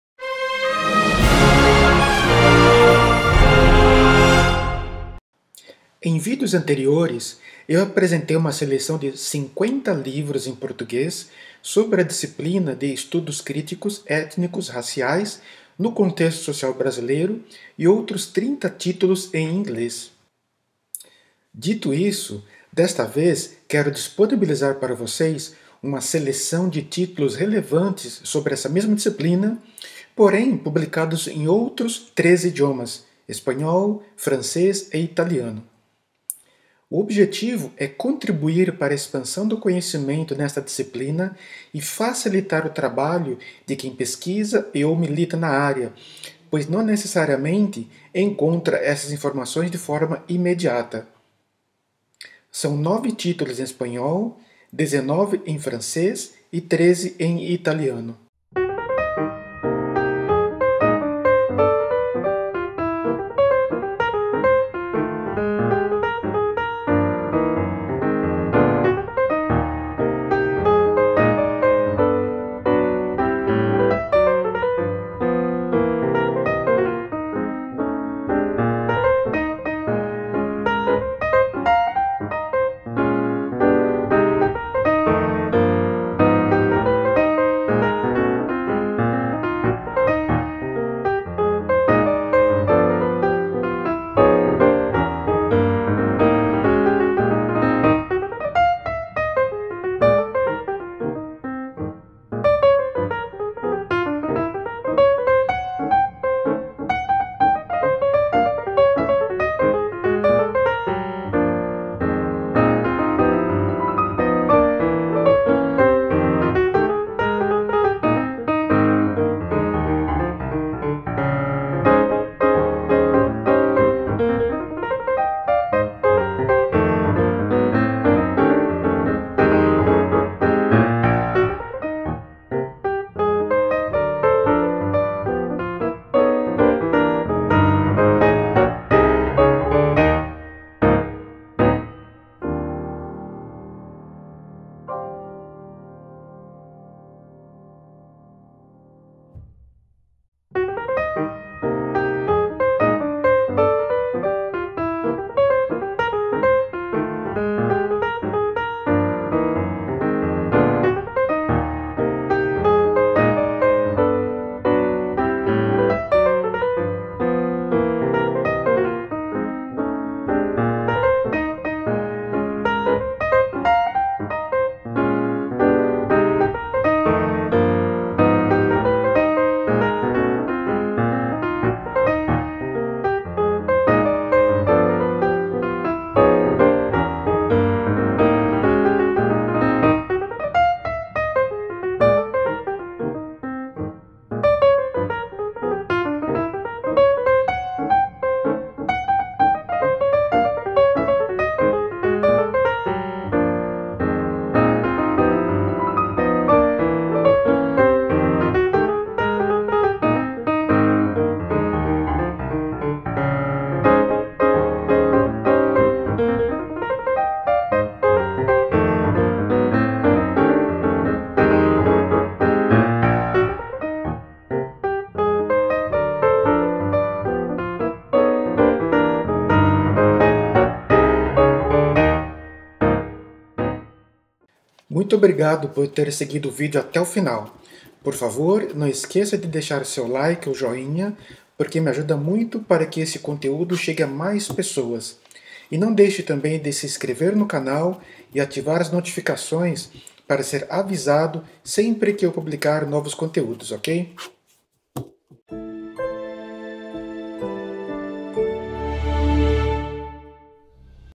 +40 Livros: Estudos críticos étnicos-raciais
Em complemento a dois outros vídeos precedentes (50 livros essenciais em português e 30 livros essenciais em inglês), este vídeo traz uma lista de +40 títulos sobre a mesma temática, porém, em três outros idiomas: Espanhol, Francês e Italiano. Este conteúdo é de interesse de estudantes, pesquisadores, jornalistas, lideranças políticas, educadores e líderes de organizações não-governamentais.